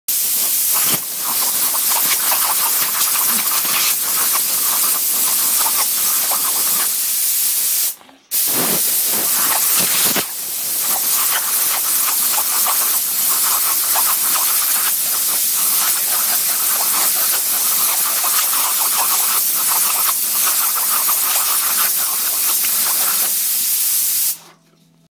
Boxer vs air compressor
My boxer loves the air compressor.